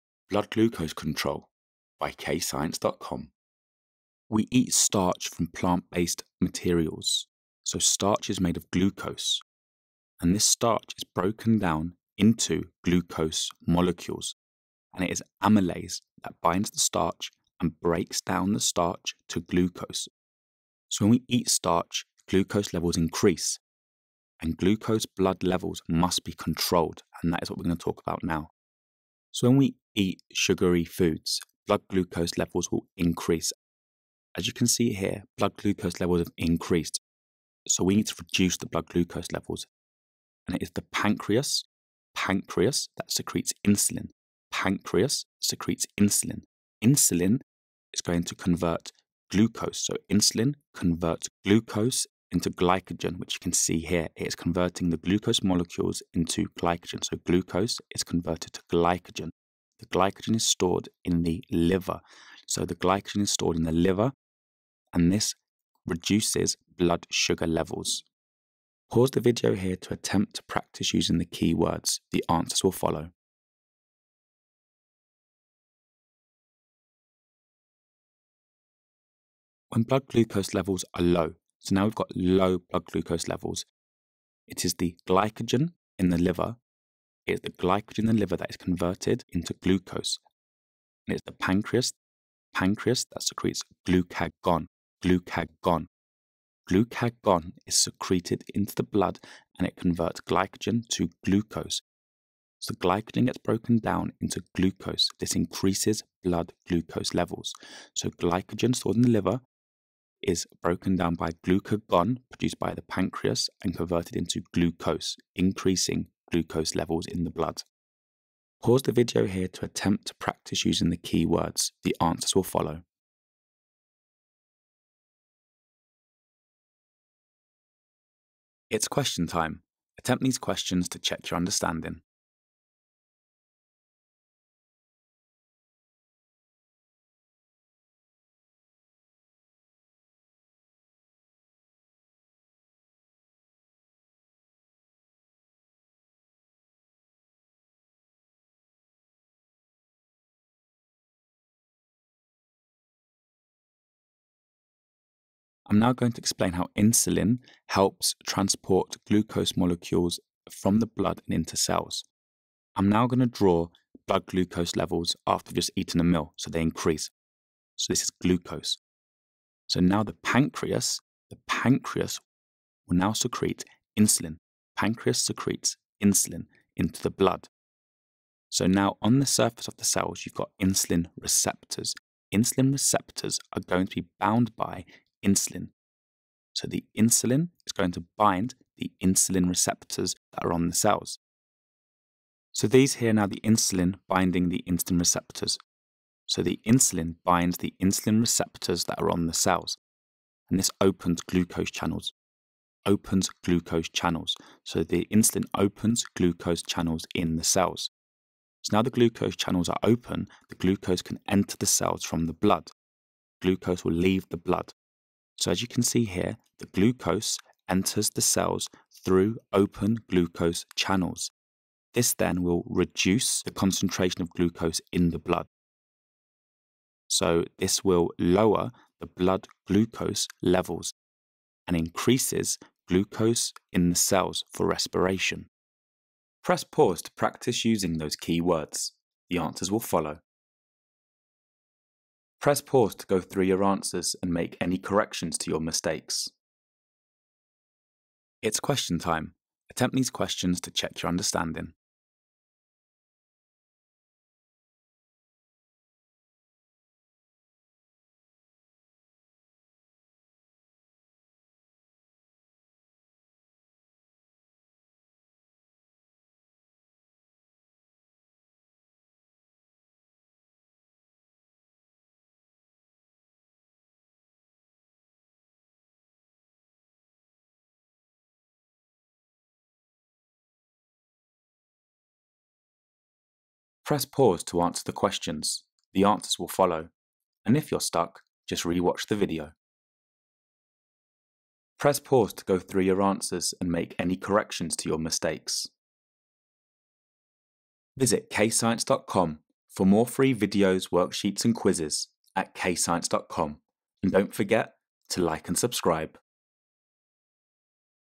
GCSE Biology - Blood Glucose Control and Insulin
When blood glucose levels get too high after a meal, the pancreas secretes the hormone insulin. The insulin that is secreted by the pancreas converts the sugar in the food called glucose into glycogen. The glycogen is stored in the liver. Because blood glucose has been converted to glycogen, this means blood glucose levels have decreased.

When blood glucose levels get too low, the pancreas secretes the hormone glucagon. The glucagon that is secreted by the pancreas converts the glycogen stored in the liver into glucose. Blood glucose levels will increase because glycogen stored in the liver has been converted to glucose by the hormone glucagon.

When blood glucose levels are too high, the pancreas secretes the hormone insulin.
The hormone insulin binds the insulin receptors on cells. When insulin binds the insulin receptors, the glucose channels in the cell membrane opens. Now that the glucose channels are open, glucose leaves the blood and enters the cell.
The glucose is used by the mitochondria for the process of respiration. This decreases the blood glucose levels.